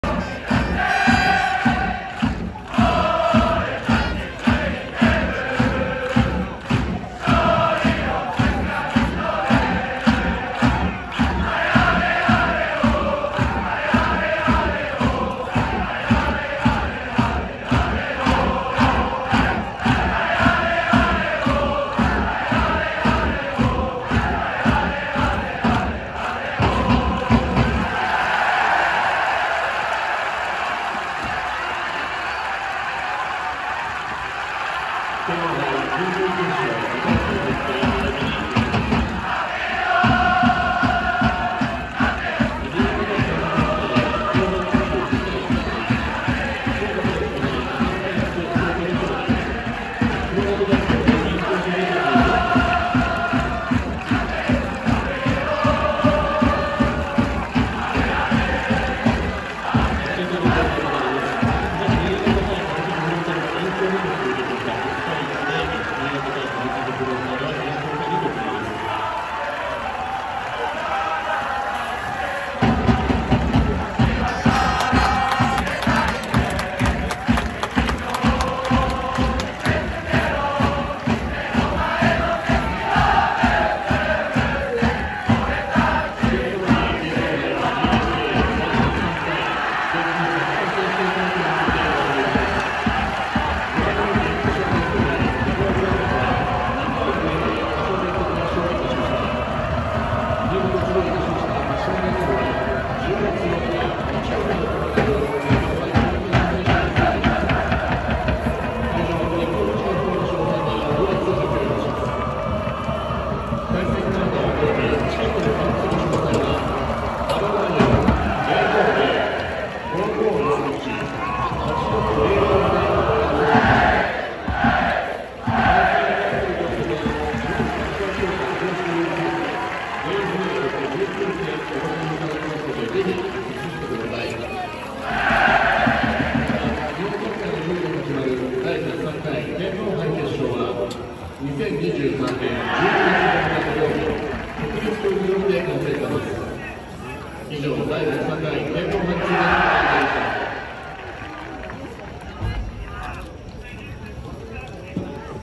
【柏レイソル】勝利チャント 天皇杯準々決勝
柏レイソル
2023/08/30 天皇杯準々決勝
柏レイソル vs 名古屋グランパス

柏レイソルの勝利後のチャント